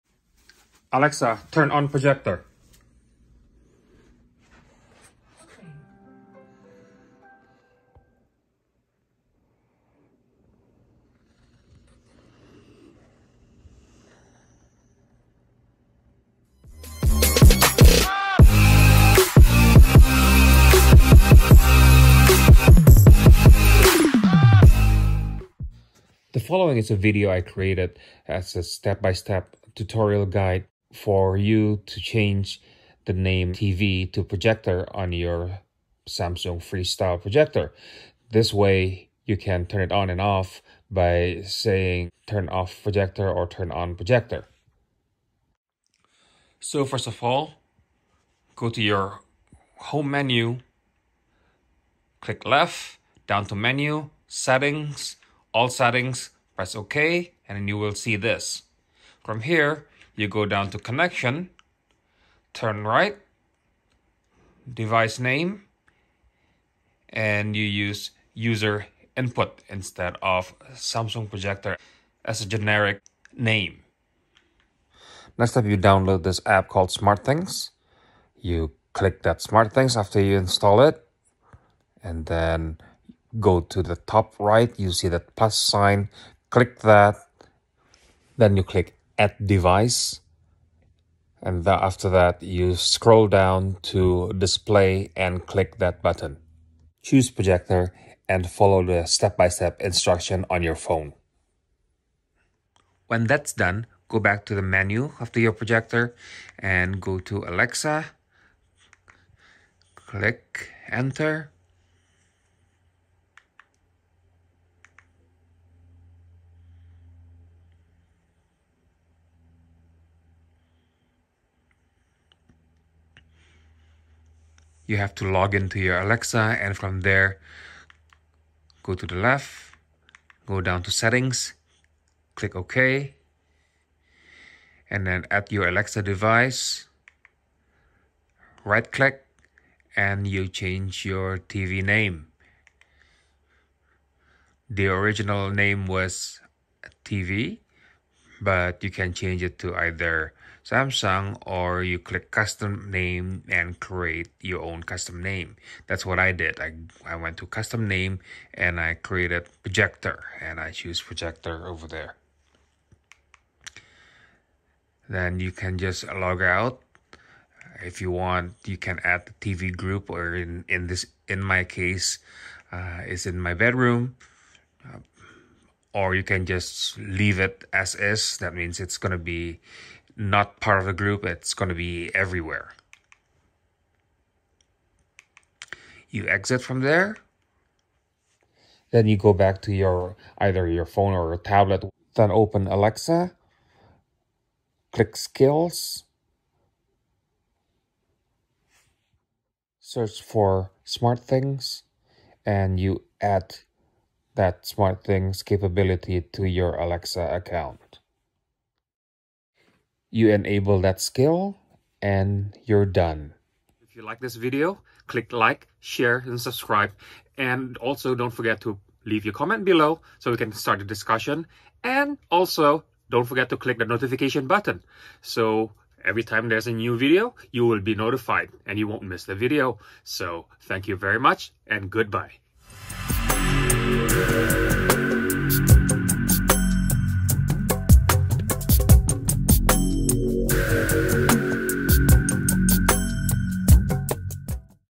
Samsung Freestyle Alexa Setup Tutorial
Controlling The Freestyle (or any Samsung displays) with Alexa Step by Step how-to tutorial

To buy The Freestyle projector: